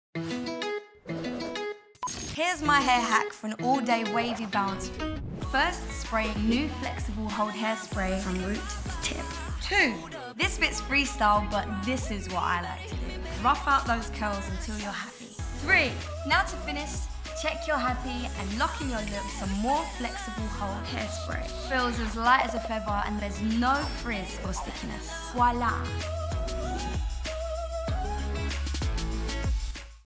Anne-Marie’s VO5 Flexible Hold Hairspray Hack
Check out how Anne-Marie uses VO5’s NEW Flexible Hold Hairspray to create an all day natural look!

For a lasting hold that won’t leave your hair frizzy, stiff or sticky get your hands on the new Flexible Hold Hairspray from VO5.